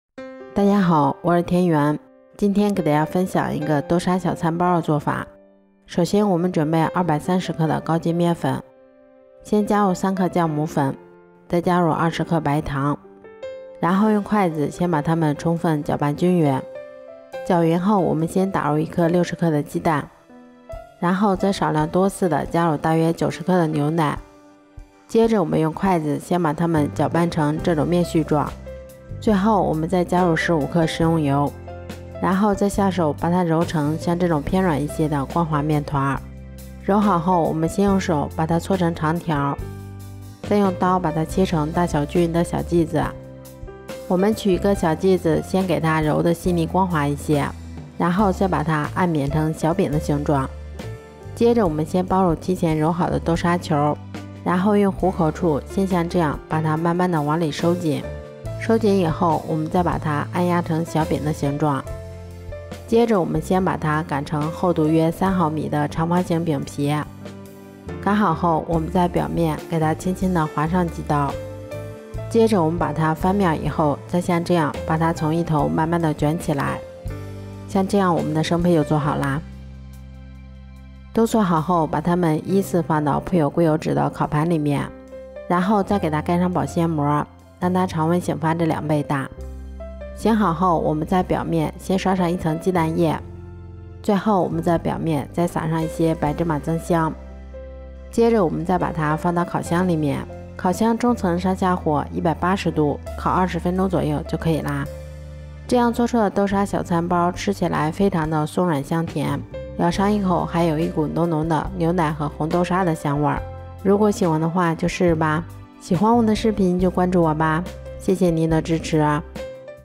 想吃面包不用买了，教你在家做，一次发酵，做法简单，孩子超喜欢
大家好，欢迎来到田园美食记。今天给大家分享一个豆沙小餐包的做法。这样做出的豆沙小餐包吃起来非常的松软香甜。咬上一口还有一股浓浓的牛奶和红豆沙的香味。喜欢的朋友们就赶快试试吧。
制作配料：230克高筋面粉，3克酵母粉，，20克白糖，1个鸡蛋，90克牛奶，15克食用油，适量豆沙馅，适量鸡蛋液，适量白芝麻
Ingredients: 230 grams of high-gluten flour, 3 grams of yeast powder, 20 grams of sugar, 1 egg, 90 grams of milk, 15 grams of edible oil, proper amount of bean paste, proper amount of egg liquid, proper amount of white sesame seeds